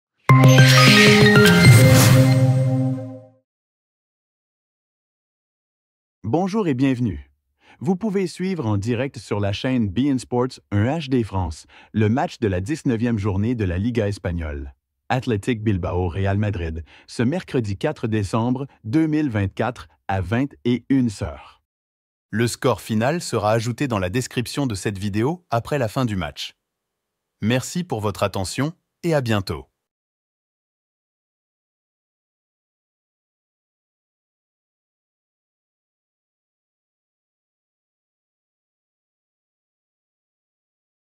LA LIGA : Bilbao 2 - 1 Real Madrid en direct sur Bein Sports 1 HD France - 04/12/2024 à 21 h
Vous pouvez suivre en direct sur la chaîne Bein Sports 1 HD France, le match de la 19e journée de la Liga espagnole : Athletic Bilbao - Real Madrid , ce mercredi 4 décembre 2024 à 21 heures.

SCORE du match : Bilbao 2 - 1 Real Madrid